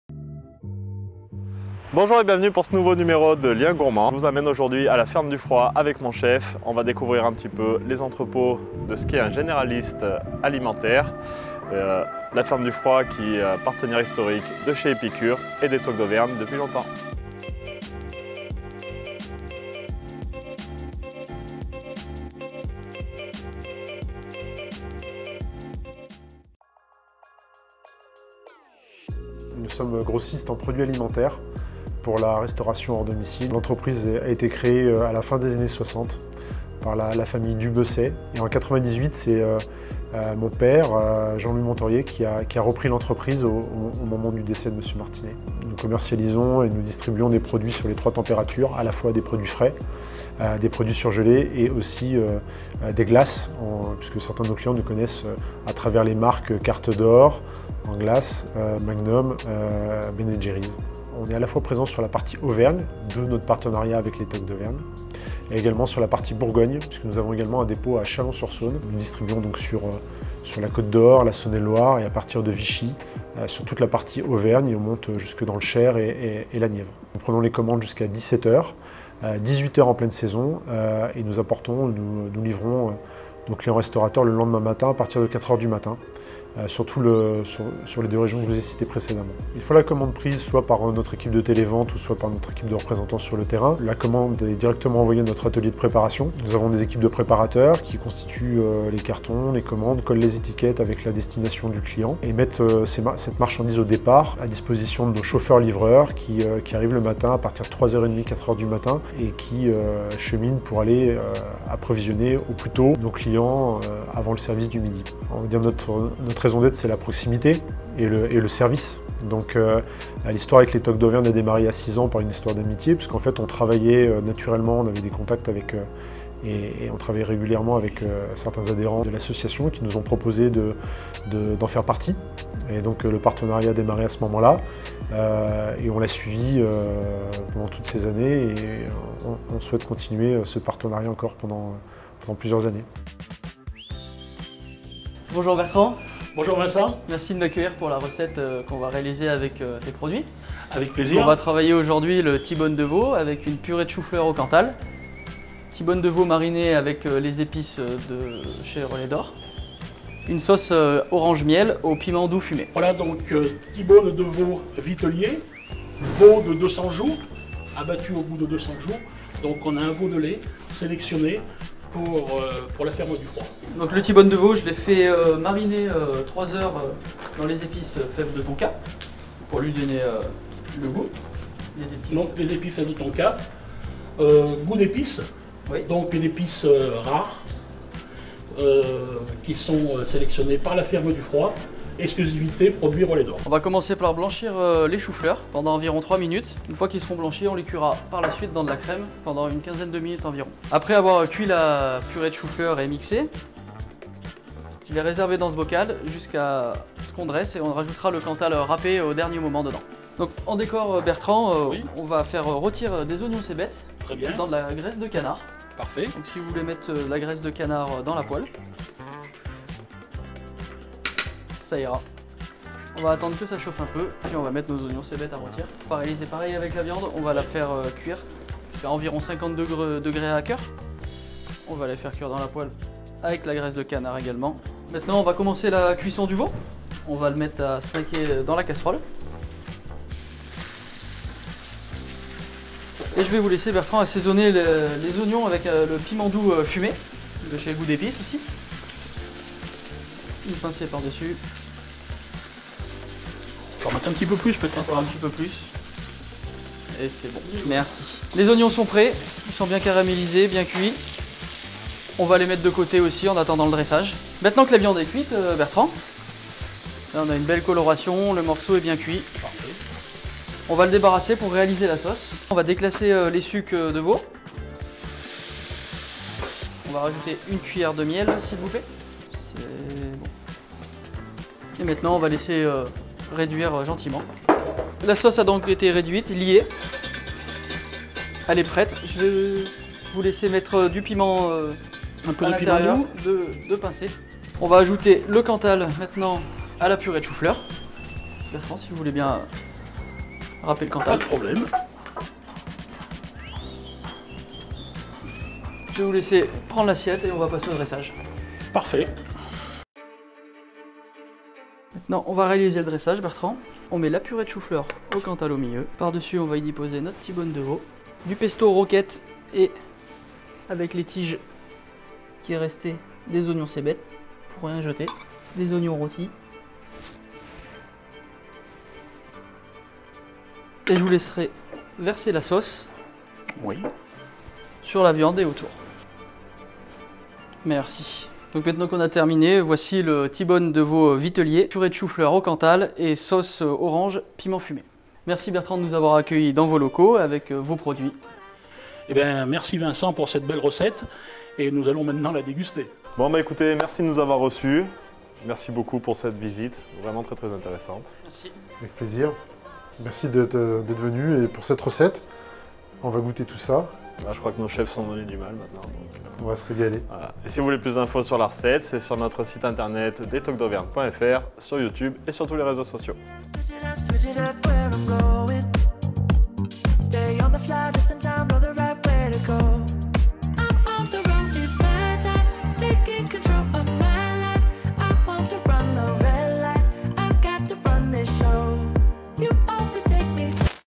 Liens Gourmands : T-bone de Veau saveur tonka, crémeux de choux fleur au cantal et sauce miel orange
La Ferme du Froid et les Toques d'Auvergne présentent : le T-bone de Veau saveur tonka, crémeux de choux fleur au cantal et sauce miel orange